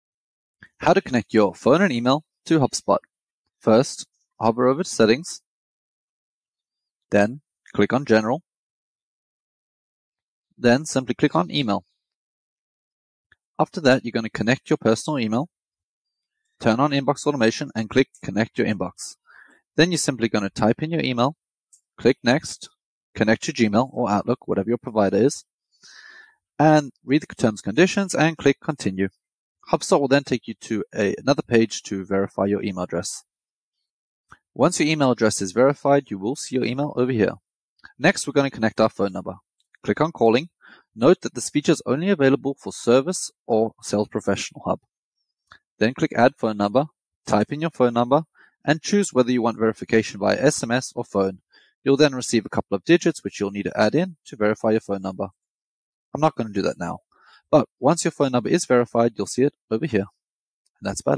How to connect your phone and email with Hubspot
In this video, learn how to quickly set up your phone and email on @HubSpot-CRM

Looking to learn more? Why not book a meeting with one of our experts